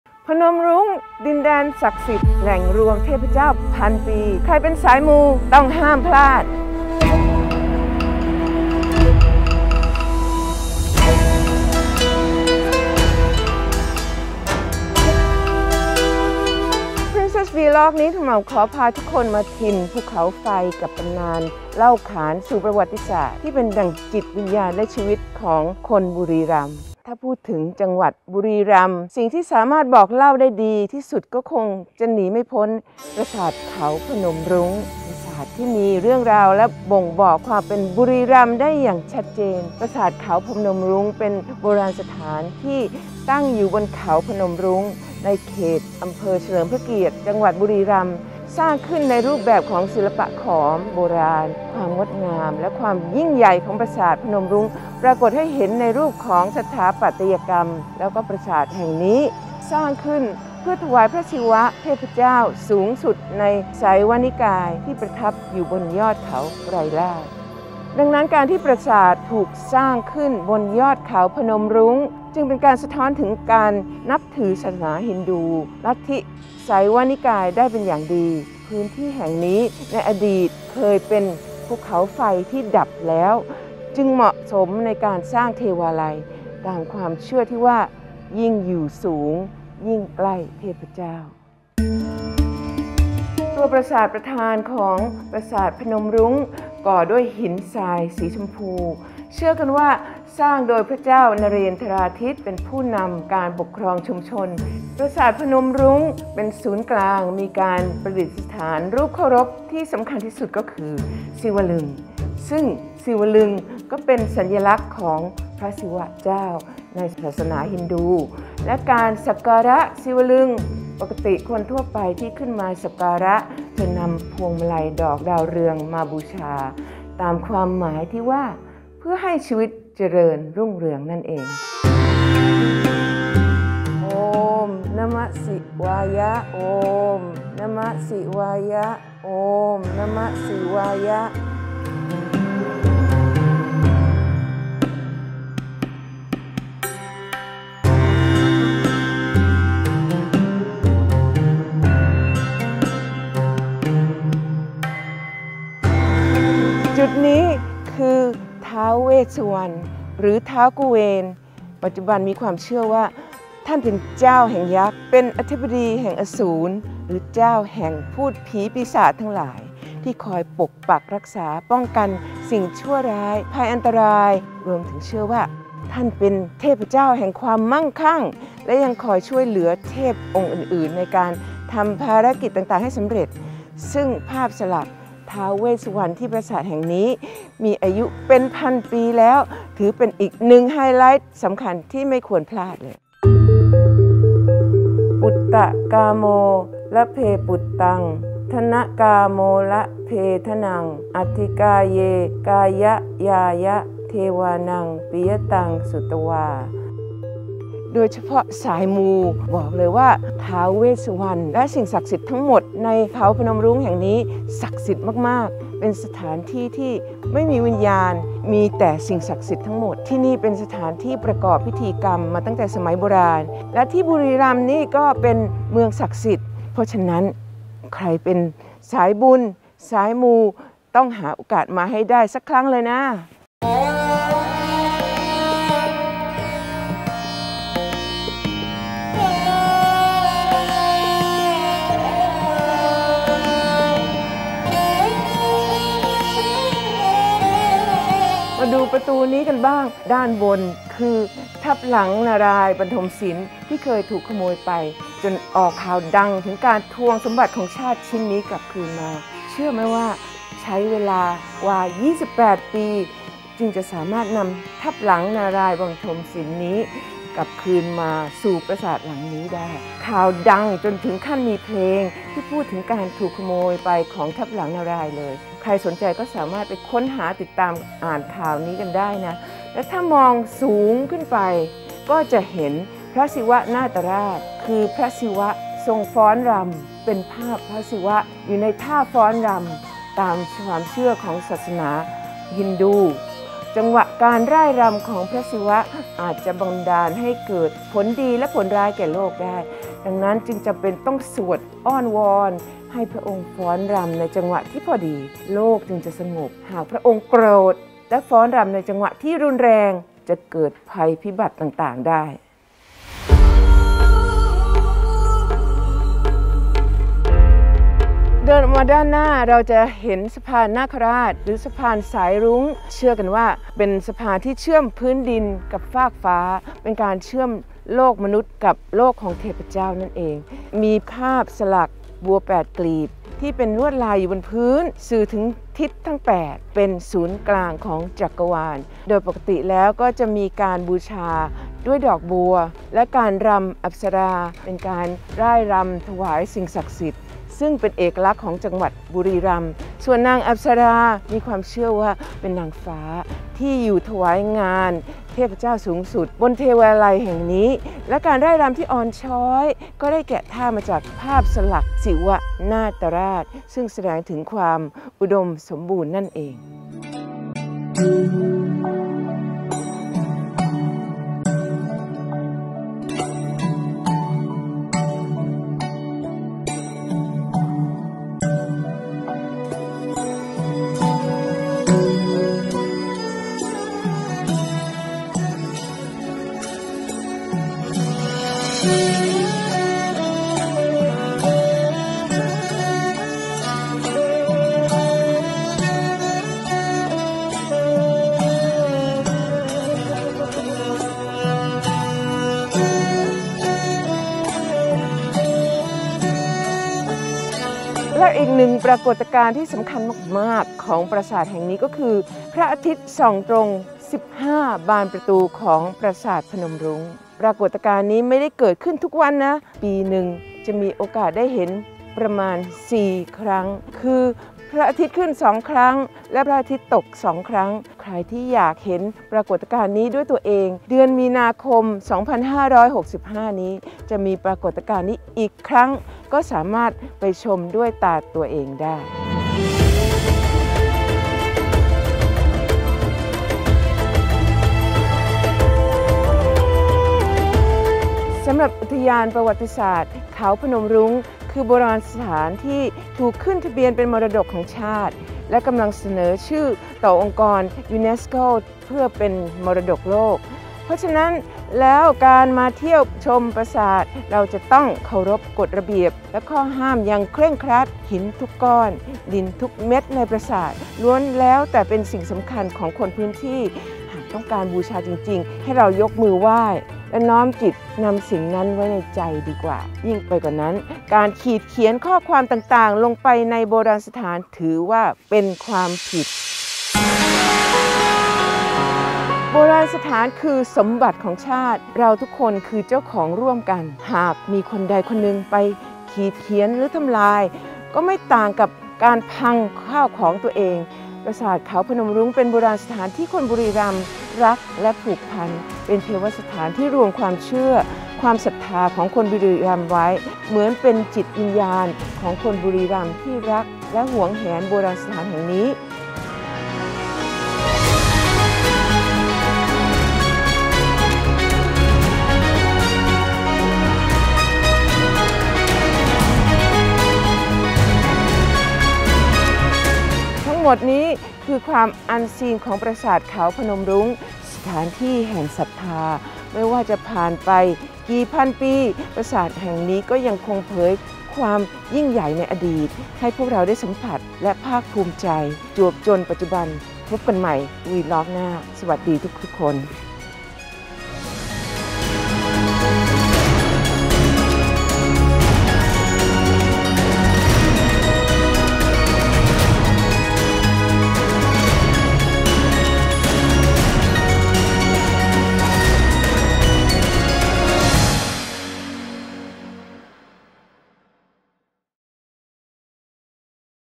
'พนมรุ้ง' ดินแดนศักดิ์สิทธิ์ l Princess Vlog Ep.41
VLOG นี้ ทูลกระหม่อมฯ ขอพาทุกคนมาชมความยิ่งใหญ่ของปราสาทเขาพนมรุ้ง ดินแดนศักดิ์สิทธิ์  แหล่งรวมเทพเจ้าพันปี  สถานที่แห่งศรัทธา กับเทวสถานที่เป็นดั่งจิตวิญญาณของคนบุรีรัมย์

ในวันที่ 5 - 7 มีนาคมนี้ ปราสาทเขาพนมรุ้งจะเกิดปรากฏการณ์พระอาทิตย์ตกตรงทั้ง 15 บานประตู ทุกคนสามารถไปชมความสวยงามนี้ได้ด้วยตาตัวเอง

ขอขอบคุณ : จังหวัดบุรีรัมย์ , อุทยานประวัติศาสตร์เขาพนมรุ้ง , ชมรม TO BE NUMBER ONE โรงเรียนประโคนชัยพิทยาคม

ช่องทางติดตาม TO BE NUMBER ONE CHANNEL